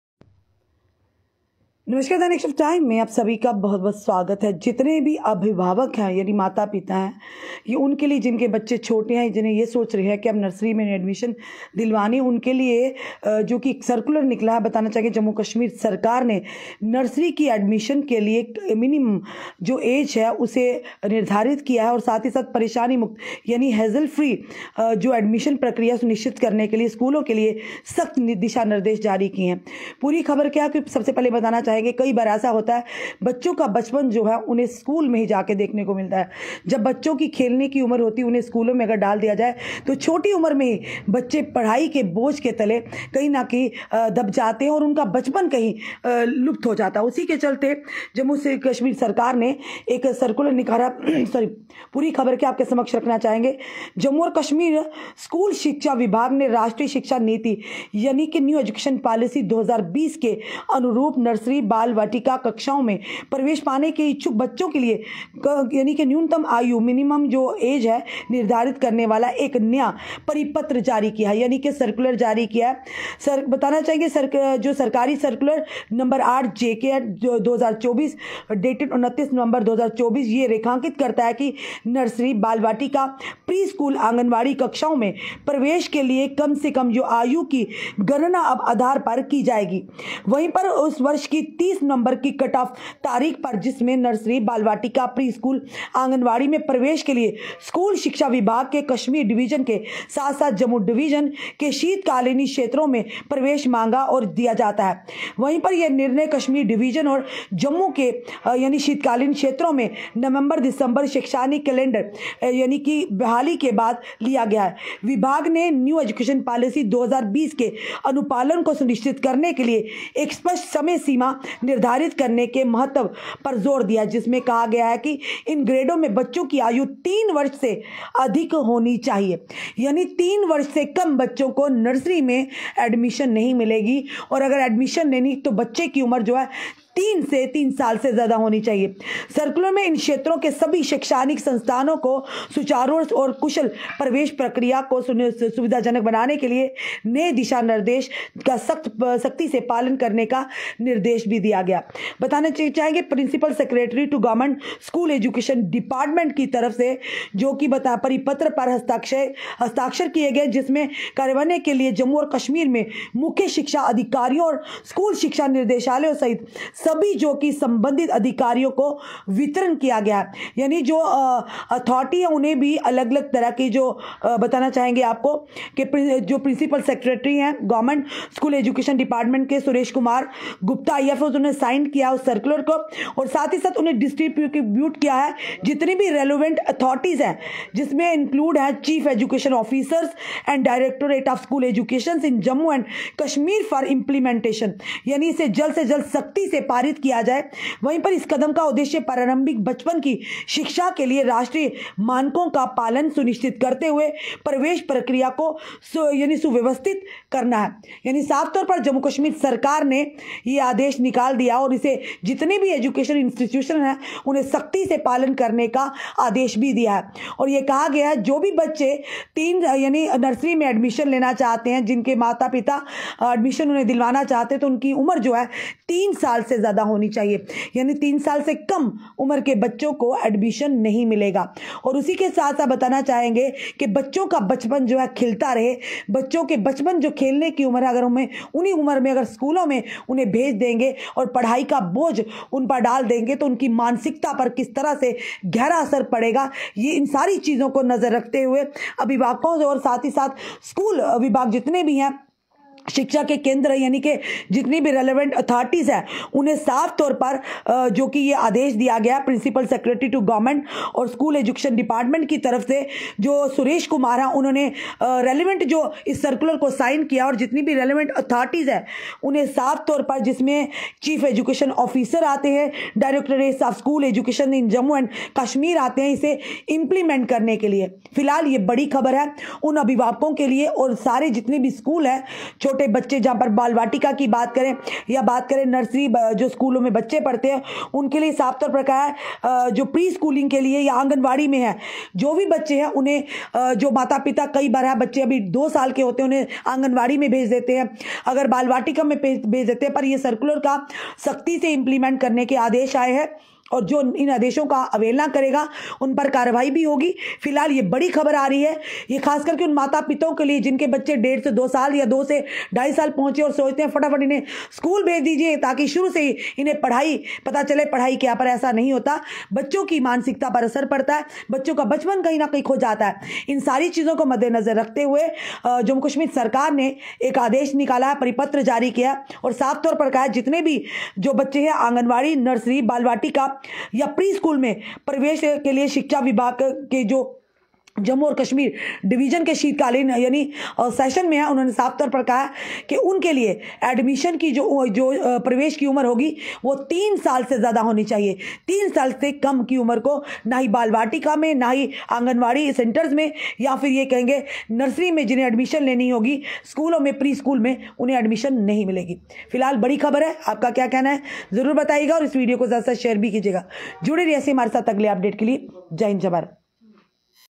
J&K Govt sets minimum age for nursery admissions, issues strict guidelines for schools to
J&K Govt sets minimum age for nursery admissions, issues strict guidelines for schools to ensure hassle free admission process